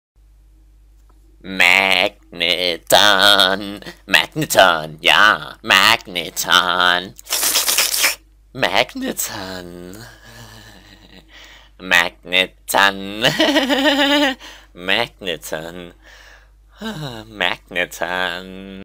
How To Pronounce Magneton
How To Pronounce Magneton is a video from PronounceGod. In this video you will learn how to say Magneton.